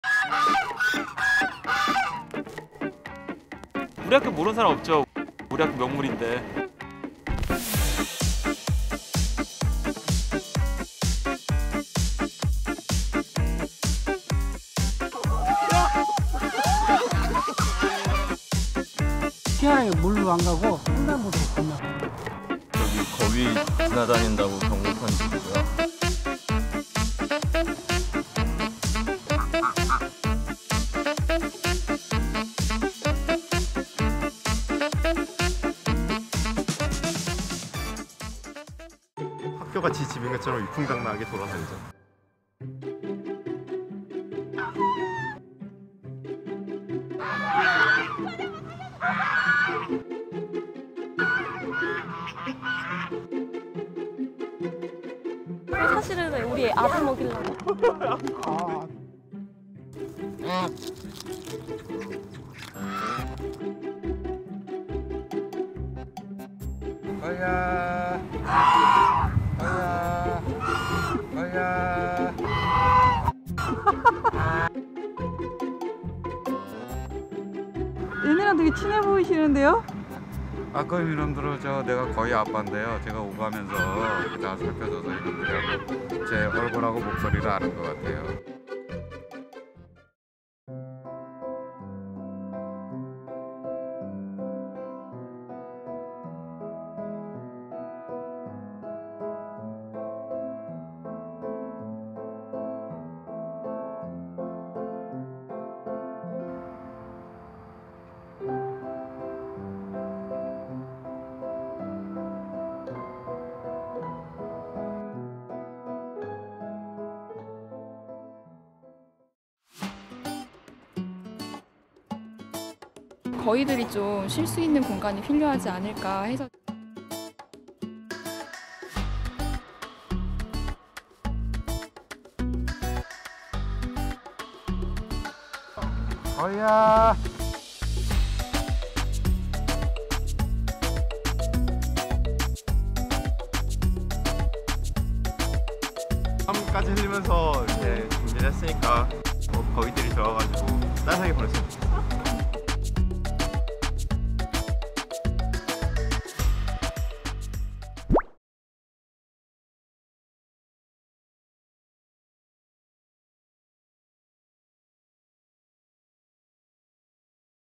“Make Way, Pay Tribute To Us” Geese Musketeers, The Rulers Of The Campus | Kritter Klub
Here comes trouble...! I mean! Our super seniors aka mascot! 7 geese musketeers hoard the lawn on campus thinking they own the entire campus to themselves!